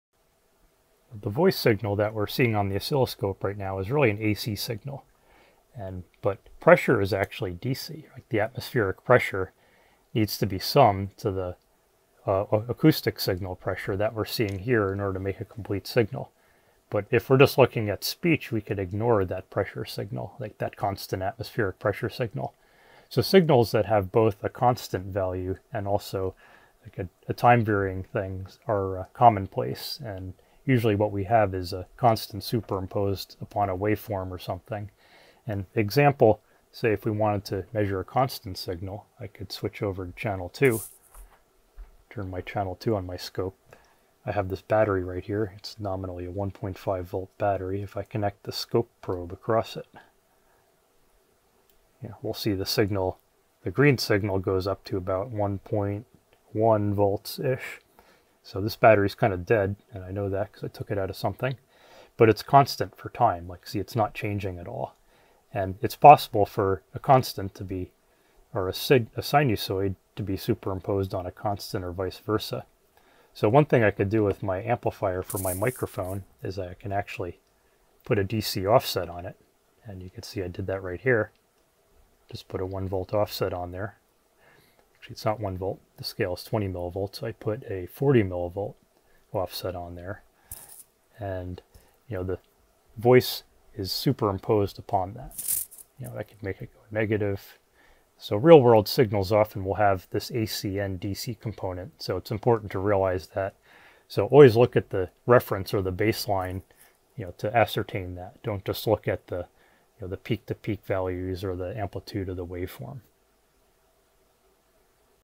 Demonstrate an AC signal superimposed on DC
Demonstrate superposition of a DC component and AC signal from a microphone.